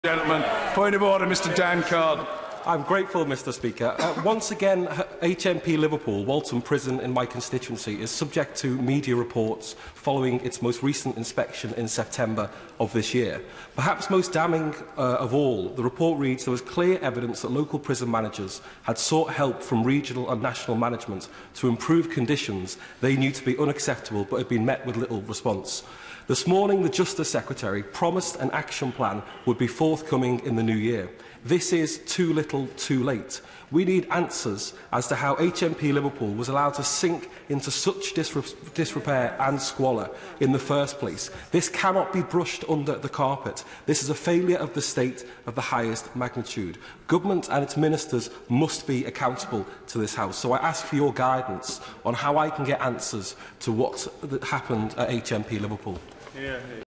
Dan Carden | We need answers on HMP Liverpool
Dan Carden MP raises a Point of Order to demand an urgent response from Government on Liverpool HMP (Walton Prison).